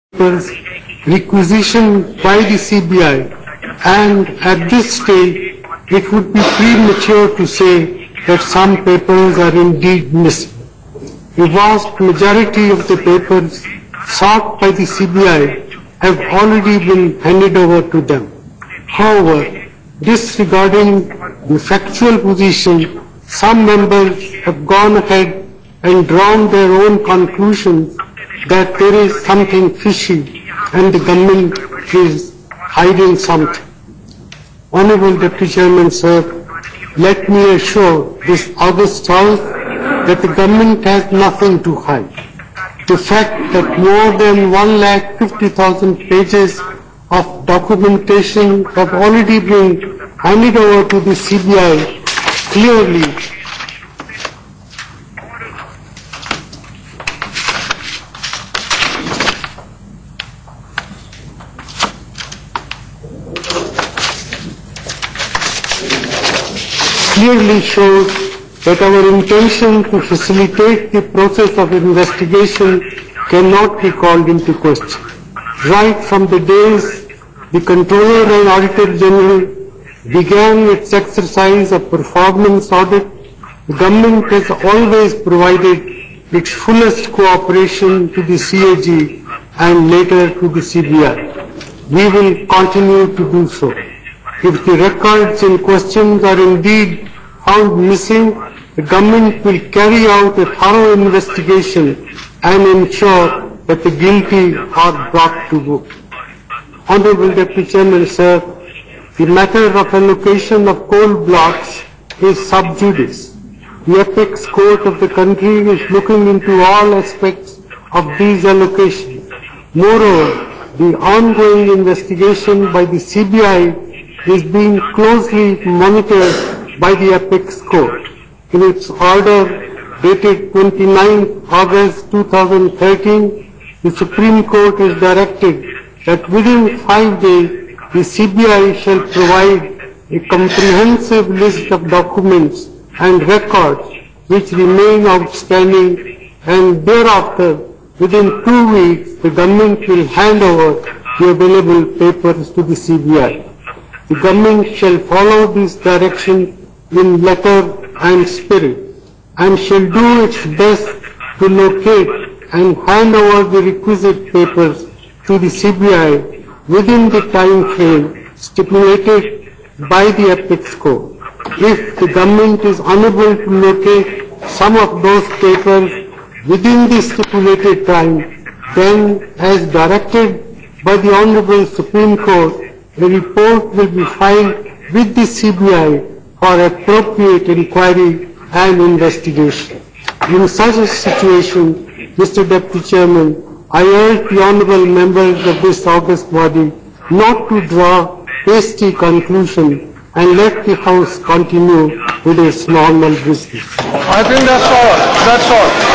Coalgate scam : Prime Minister's statement in the Lok Sabha on missing coal files
New Delhi: Prime Minister Manmohan Singh today made a statement in both the houses on the missing coal files.

With regard to the so-called missing files or papers pertaining to the ongoing investigation into allocation of coal blocks, I would like to emphasize that the Government is making all efforts to locate the papers requisitioned by the CBI and, at this stage, it would be premature to say that some papers are indeed missing.